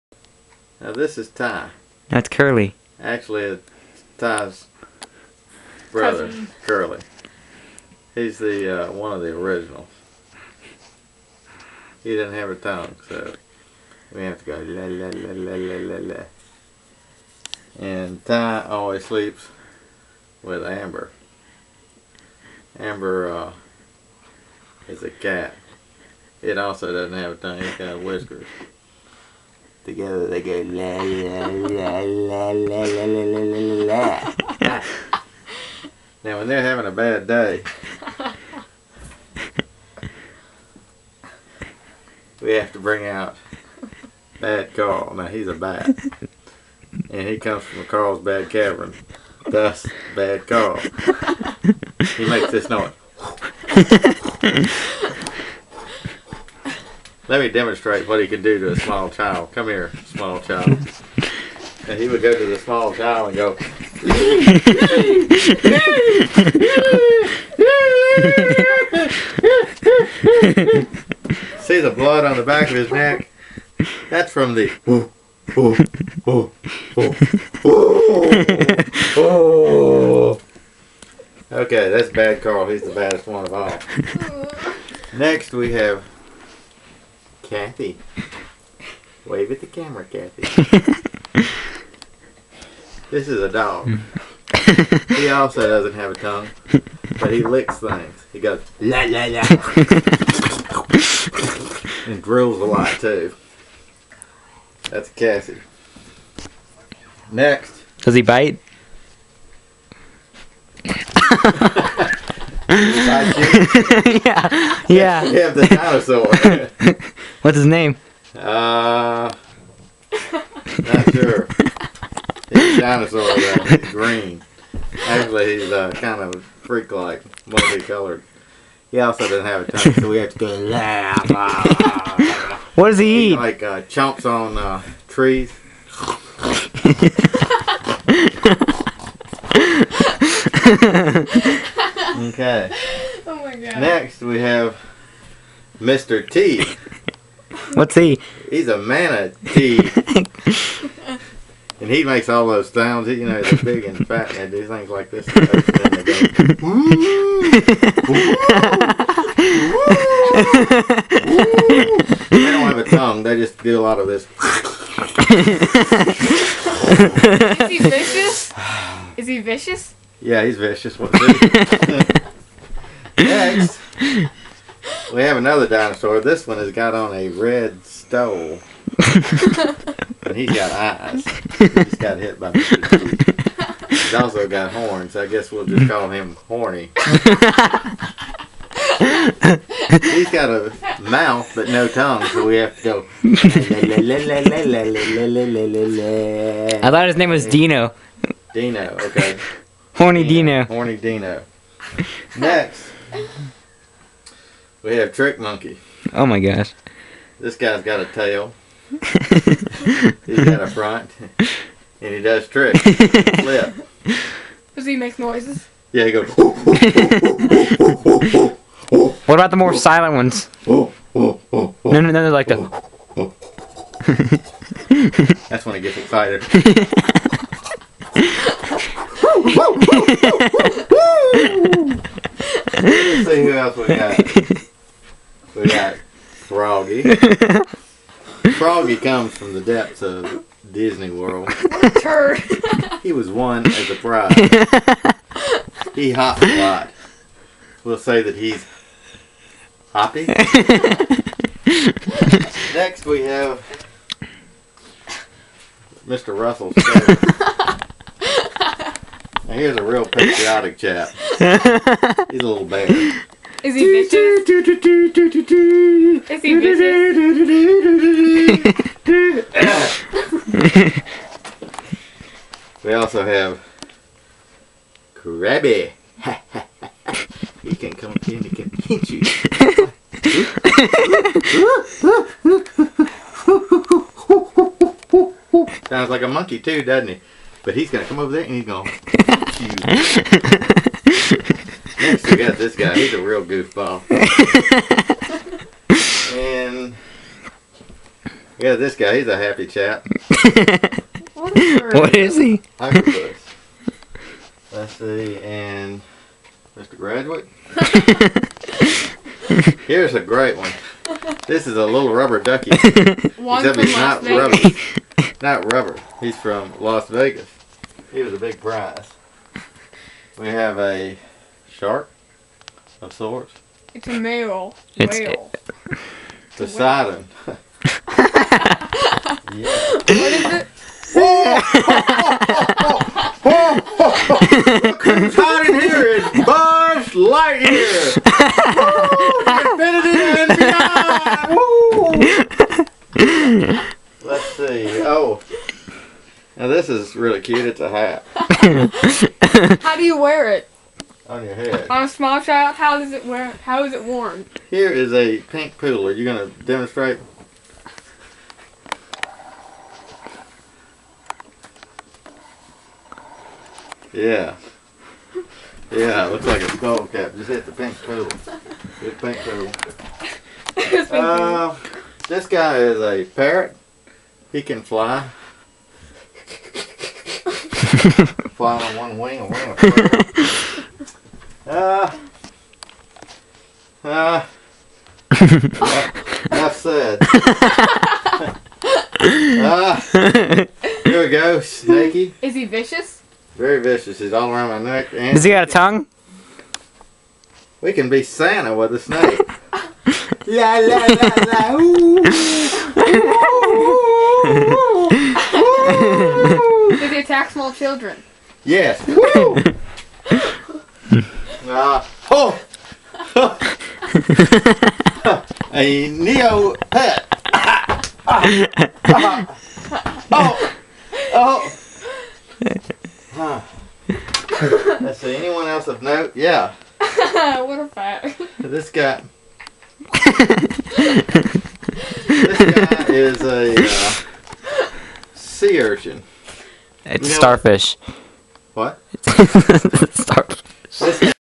Bedtime Animals
My nightly performance with stuffed animals to get the kids ready for bed.